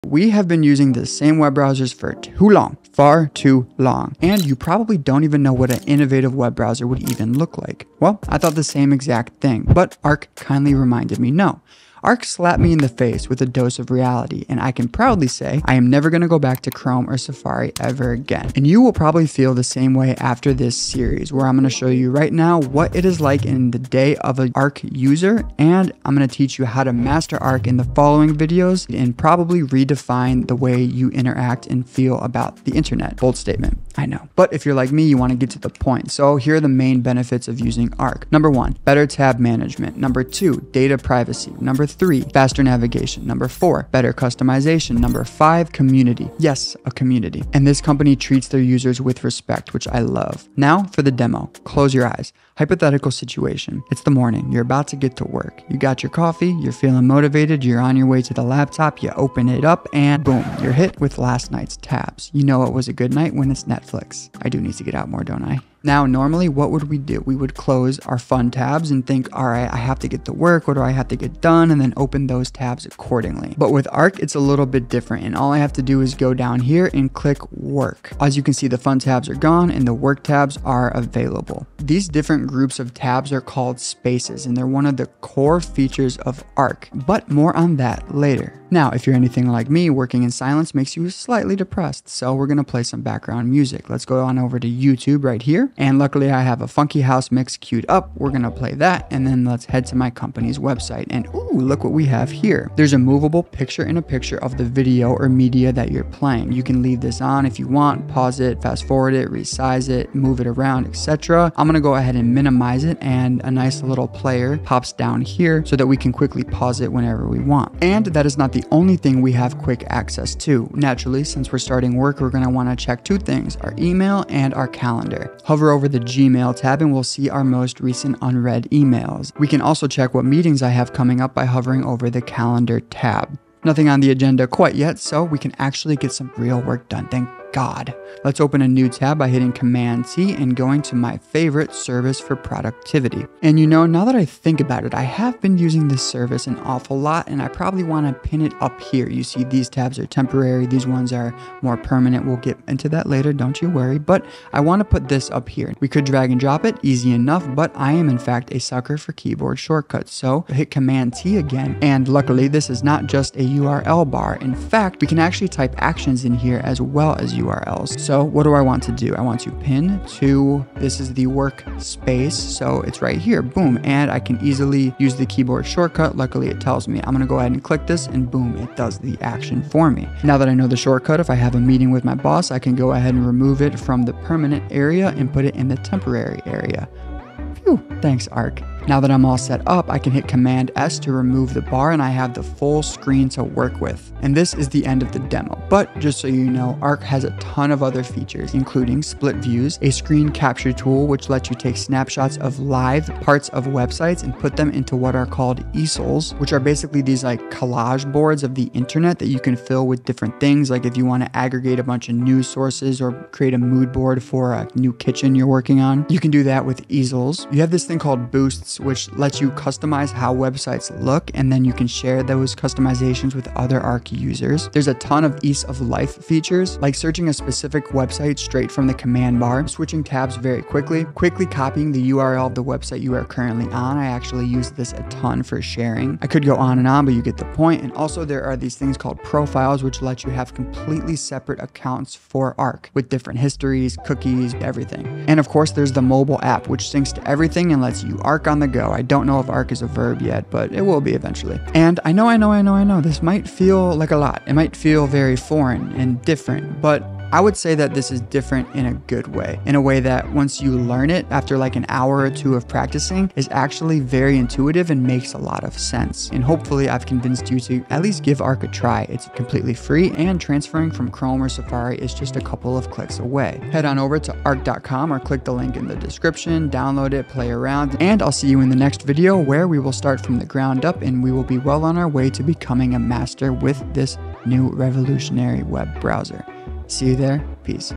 Complete Guide to Arc - Why Arc?
Part 1 of the 4 part tutorial series on mastering Arc and redefining how you interact with the internet. Welcome aboard 💻⛵️🌎.

📚 Video Chapters 📚
0:00 Intro to the course
0:34 TLDR: Why Arc?
0:50 Demo - The beauty of “Spaces”
1:29 Mini Picture-in-Picture Player
1:57 Quick Look (emails and calendar)
2:19 The URL bar and Pinned tabs
3:15 Other Awesome Arc Features
4:15 Conclusion